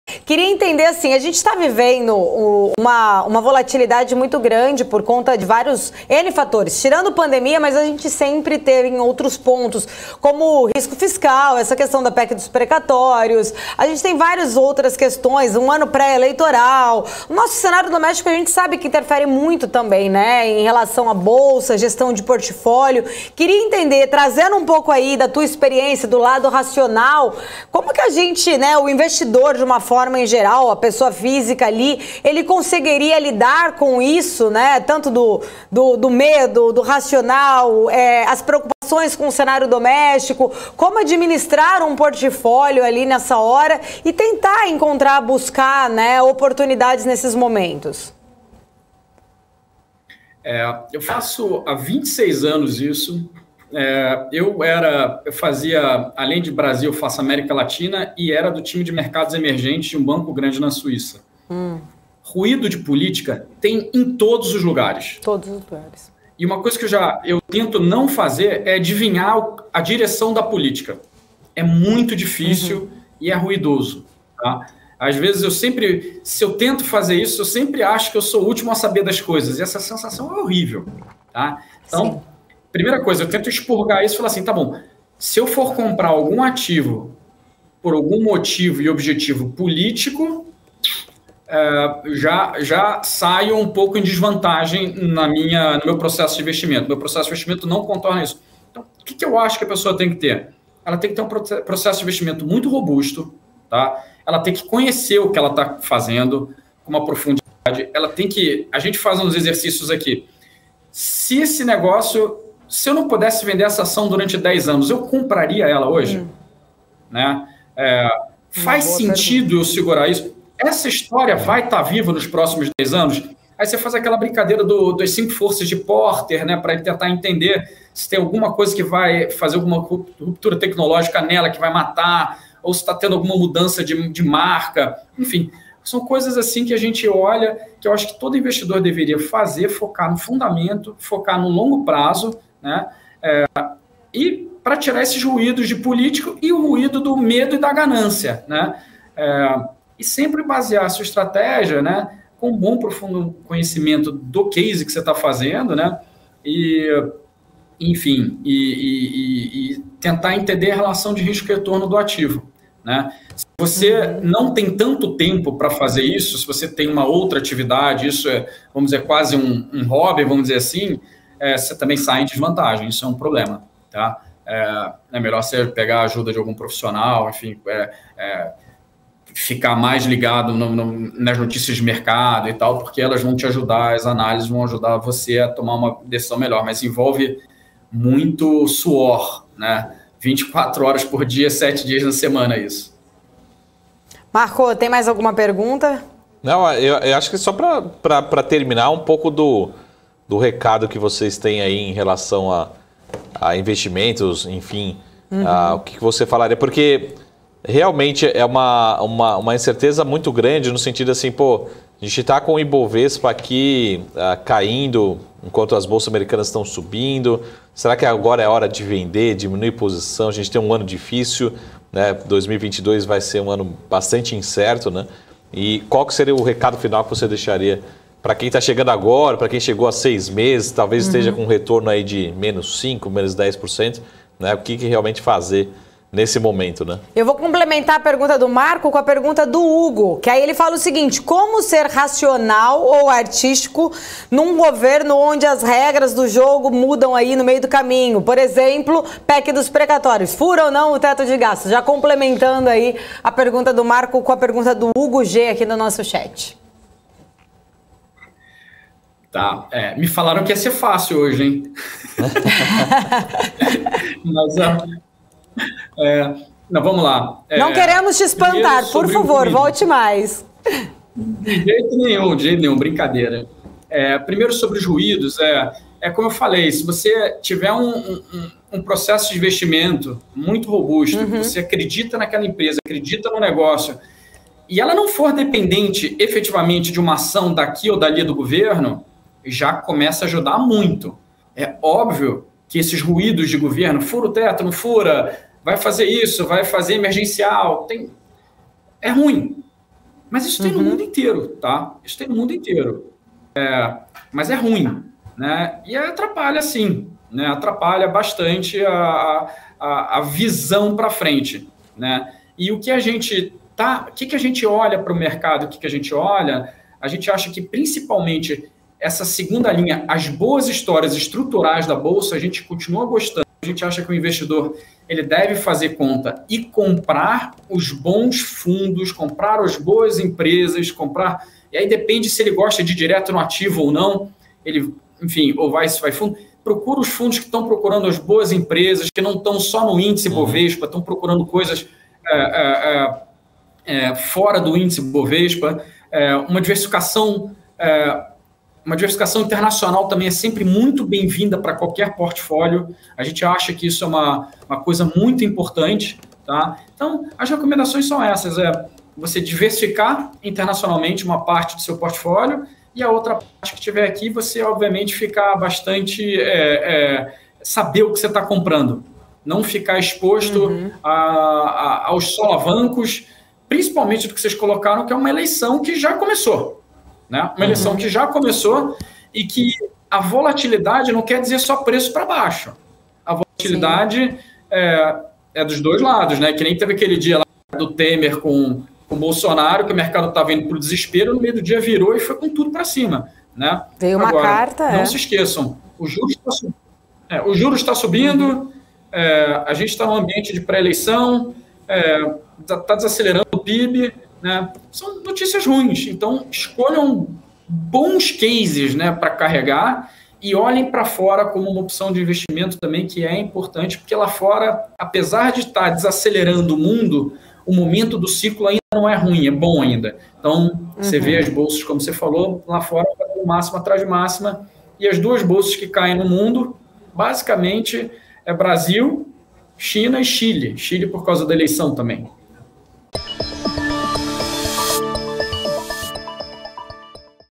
Como encontrar oportunidades em cenário de risco? CIO de ações avalia
Roberto Chagas, CIO de ações da EQI Asset Management, dá dicas para investidores manter a racionalidade diante de queda das ações e volatilidade do mercado.